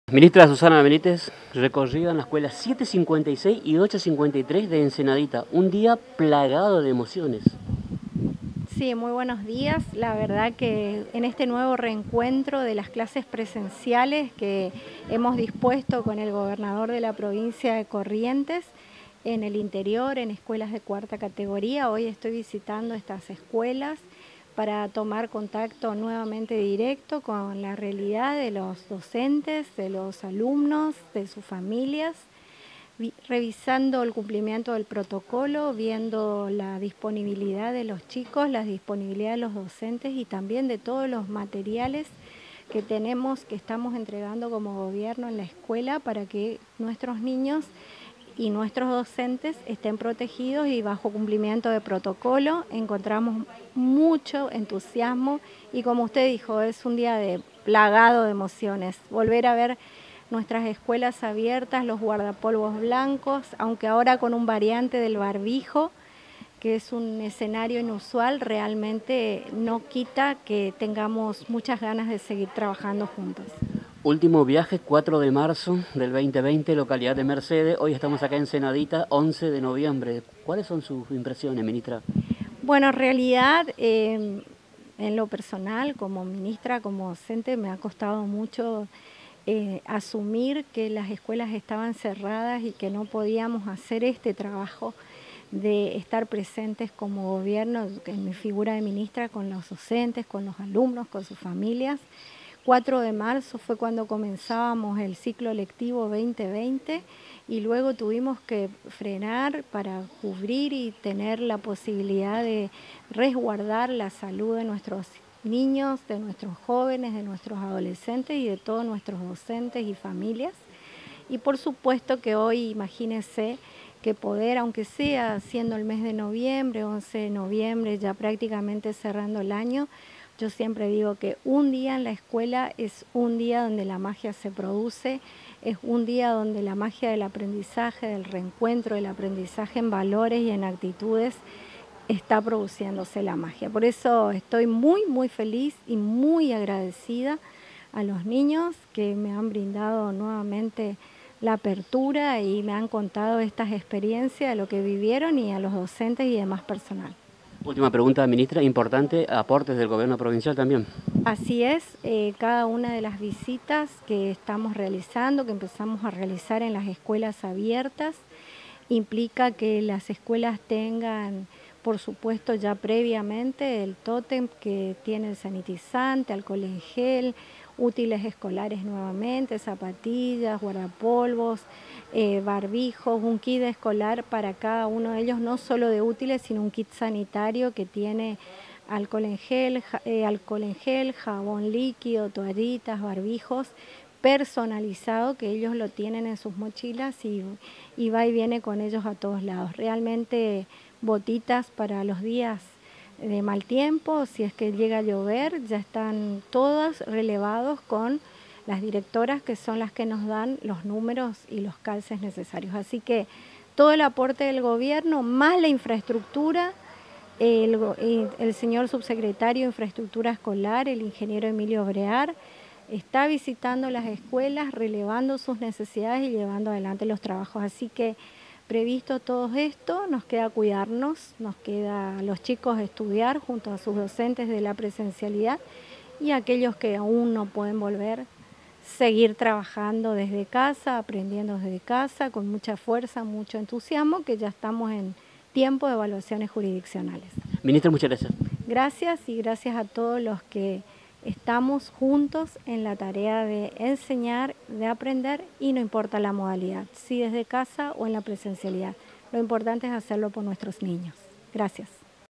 Recorrida a escuelas primarias de 4ta categoría
Lic. Susana Benítez - Ministra de Educación
Visita a la Escuela Nro 756  y Escuela Nro 853 en Ensenadita - San Cosme